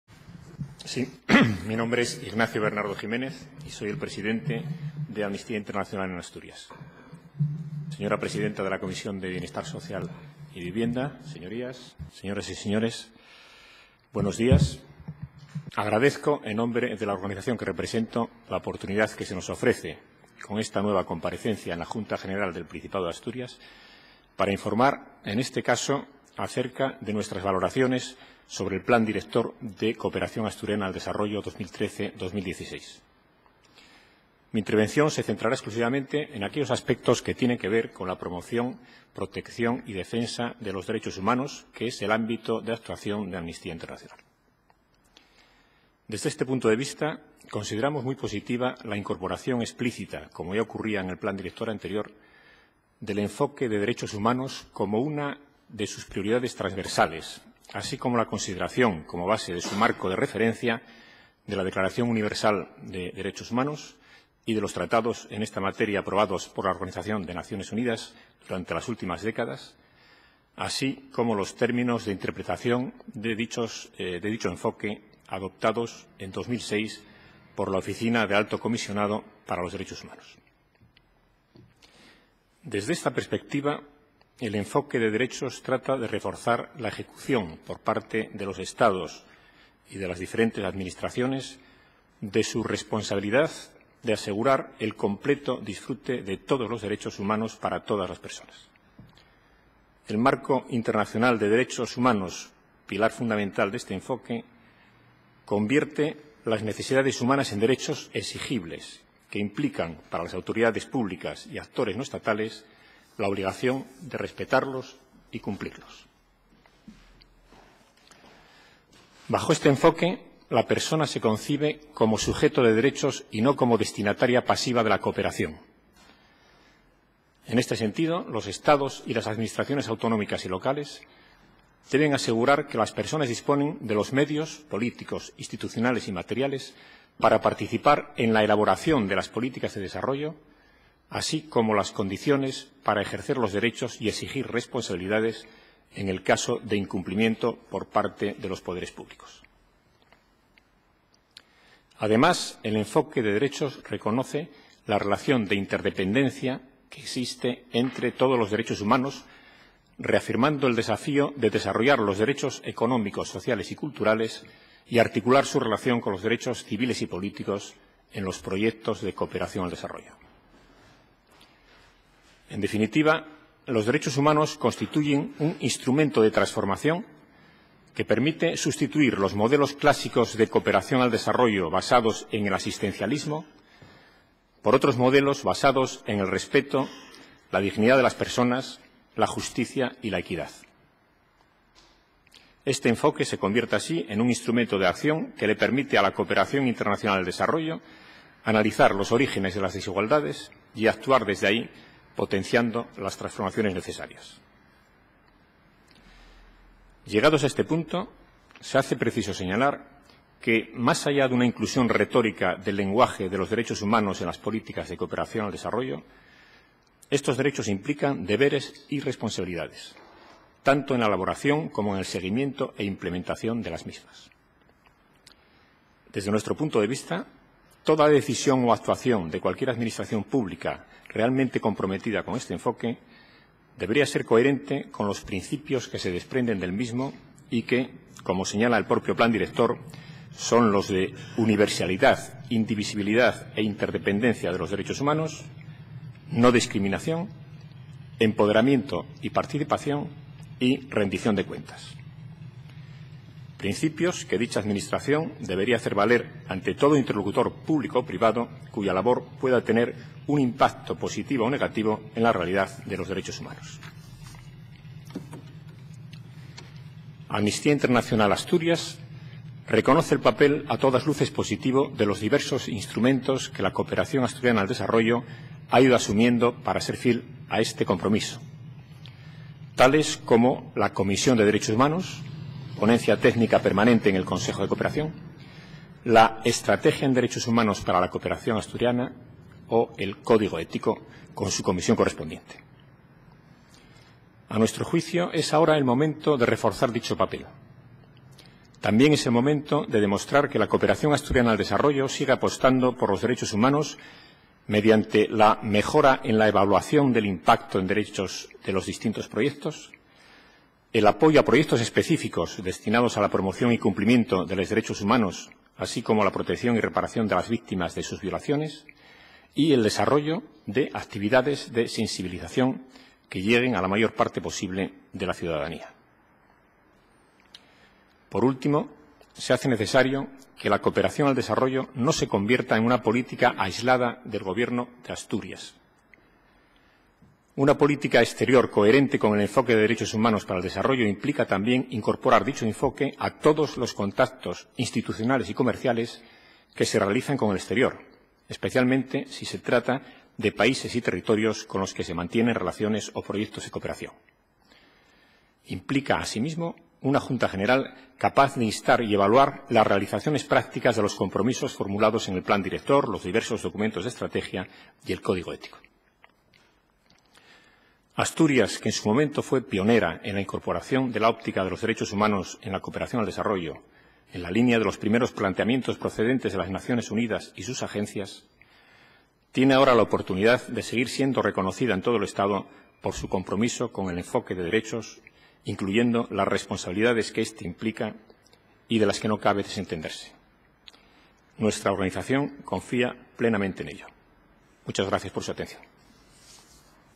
Comparecencia AI en la JGPA 10 11 2014 09 09
Intervención del presidente de Amnistía Internacional Asturias, Ignacio Bernardo Jiménez, en la comisión de Bienestar Social y Vivienda de la Junta General del Princiapdo de Asturias el 10 de noviembre de 2014. Su participación formó parte de unas comparecencias informativas acerca del Plan Director de Cooperación Asturiana al Desarrollo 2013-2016, y fue a propuesta del Grupo Parlamentario Mixto. AI Asturias considera muy positiva la incorporación explicítca del enfoque de derechos humanos como una de lasprioridades transversales del plan y en esta intervención recuerda que es preciso tener en cuenta que el enfoque de derechos humanos implica unos deberes y responsabilidades, que no debe quedarse en una inclusión retórica del lenguaje.  AI indicó que es necesario que este enfoque no se aplique solo en la cooperación al desarrollo sino que debe tenerse presente en todos los contactos institucionales y comerciales que el gobierno asturiano realiza con el exterior.